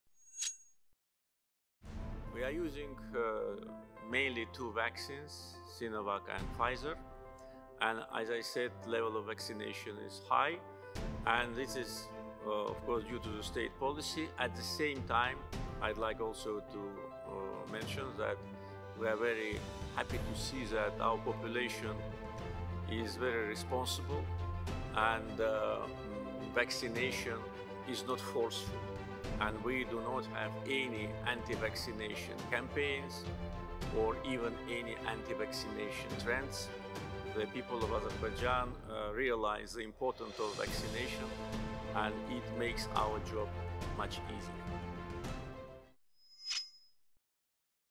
The people of Azerbaijan realize the importance of vaccination, and it makes our job much easier.
Ilham Aliyev: "We do not have any anti-vaccination campaigns or even any anti-vaccination trends. The people of Azerbaijan realize the importance of vaccination, and it makes our job much easier".